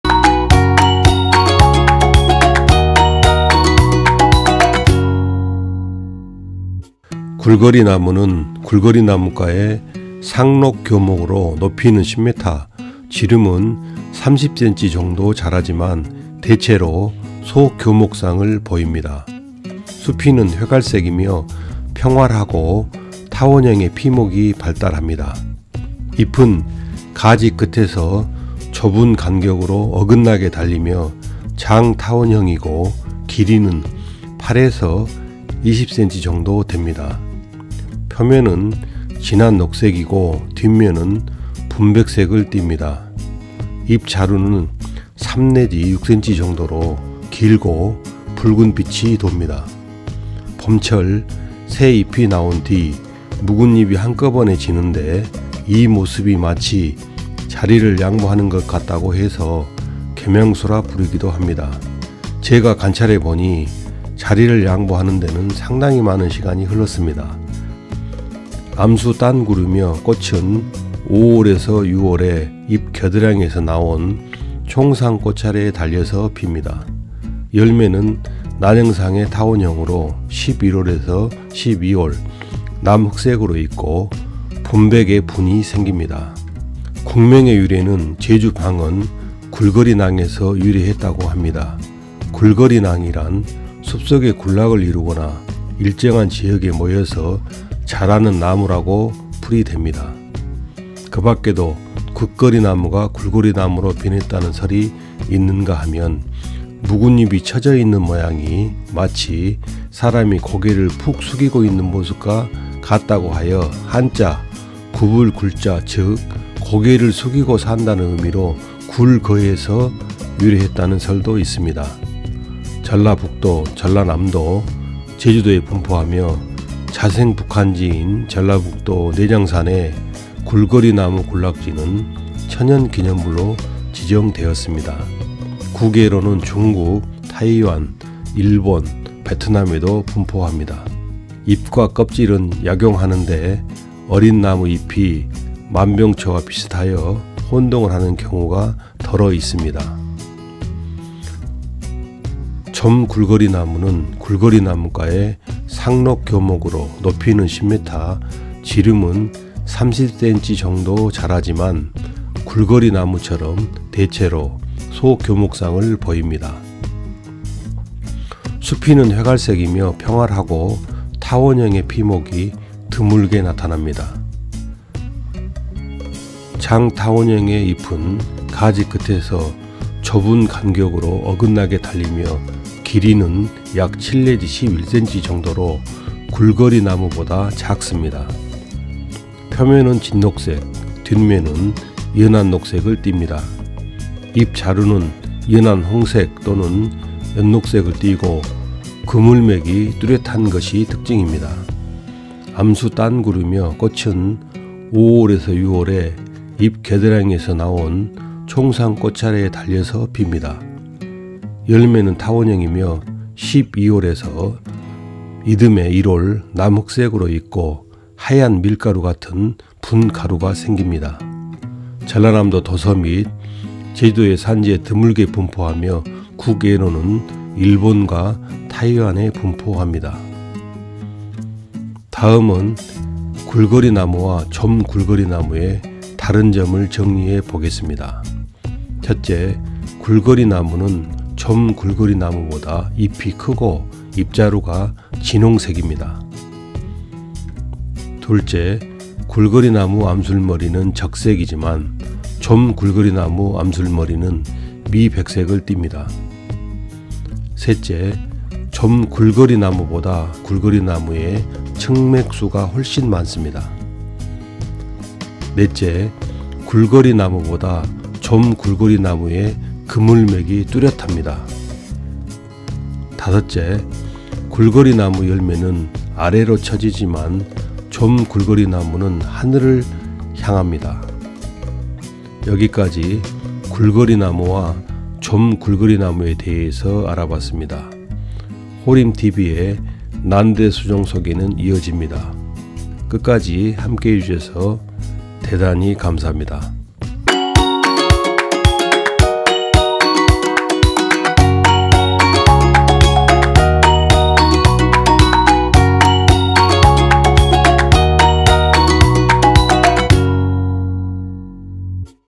정원수로 좋은 굴거리나무와 좀굴거리나무, 숲 전문 HORIM-TV
굴거리나무와 좀굴거리나무 동영상입니다.
★Timestamps★
00:00 인트로
00:10 굴거리나무
02:35 좀굴거리나무
03:54 굴거리나무와 좀굴거리나무의 다른점
04:57 아웃트로